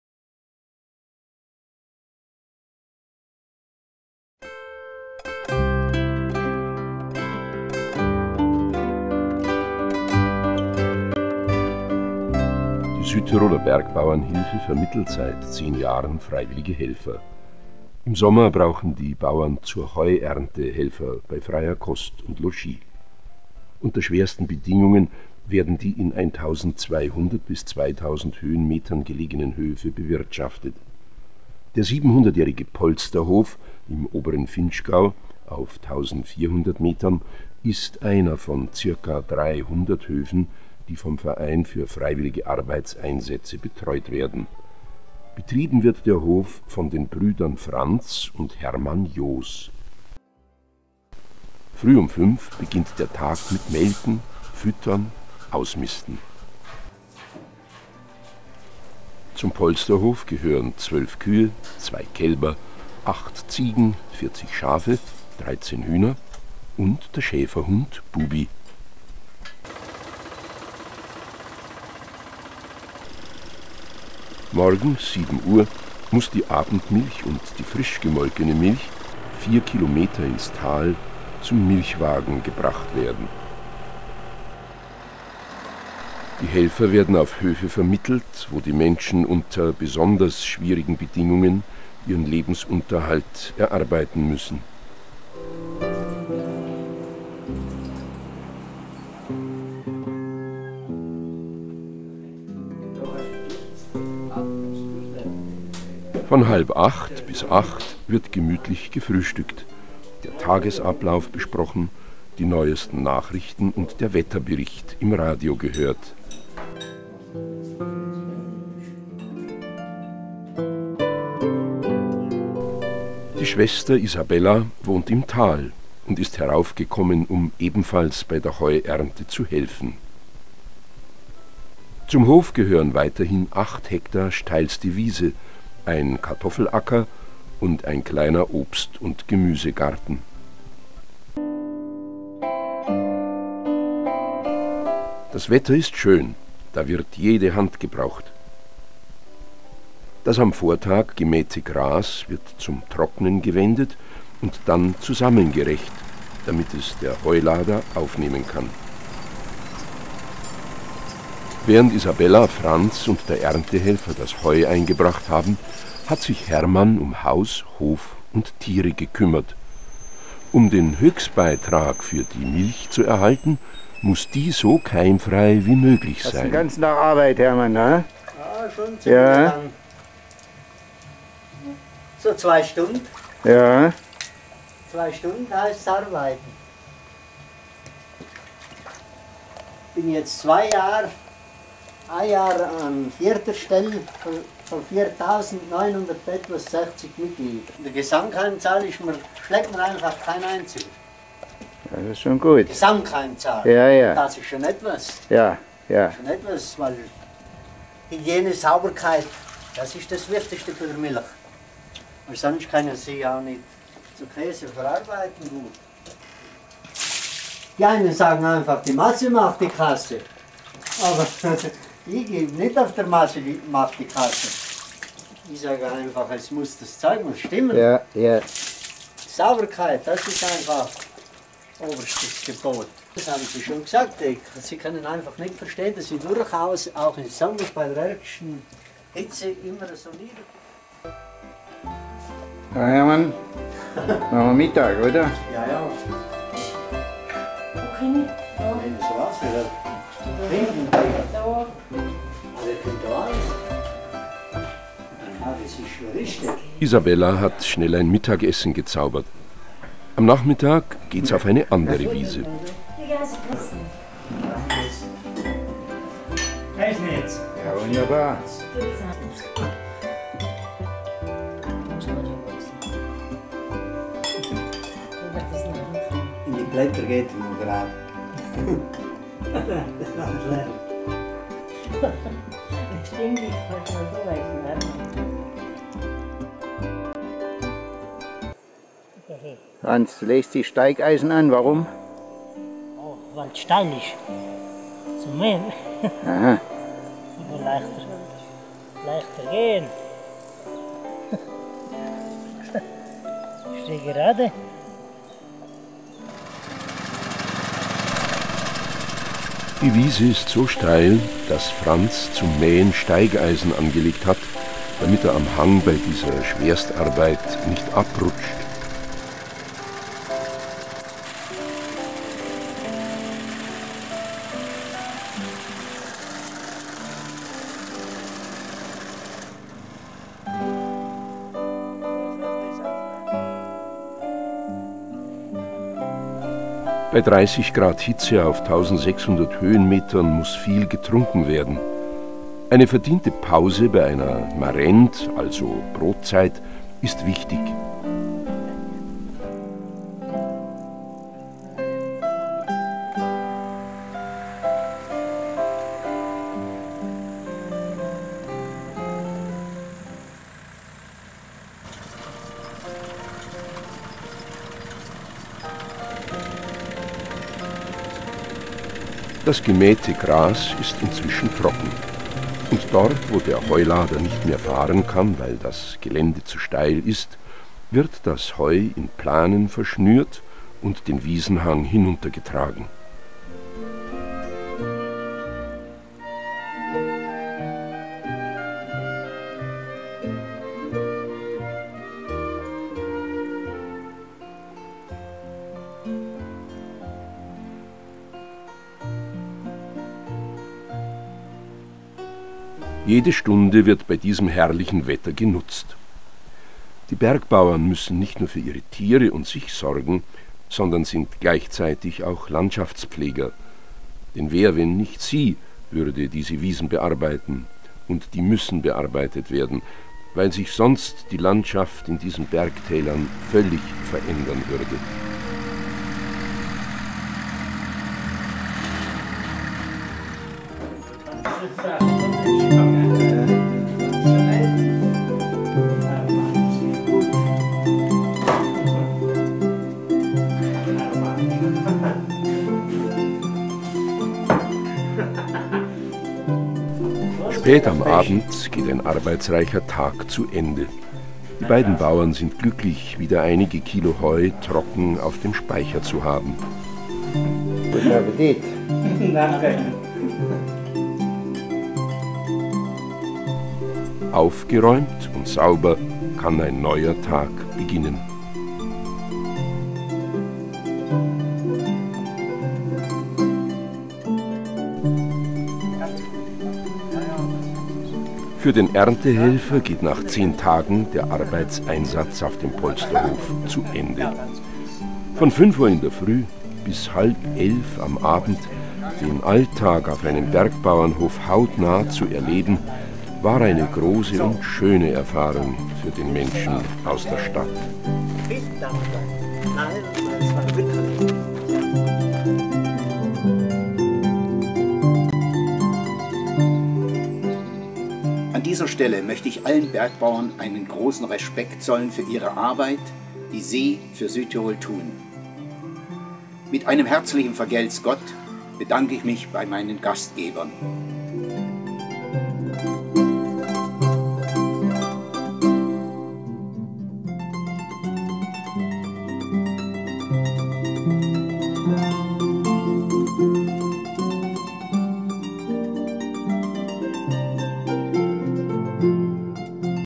"Bergbauern in Südtirol" einen Tag aus dem Leben der Bergbauernbrüder Franz+Hermann
Anstatt Urlaub in Südtirol, diesmal als Erntehelfer auf einem Bergbauernhof im oberen Vinschgau den Brüdern Franz und Hermann Joos bei der Heuernte geholfen.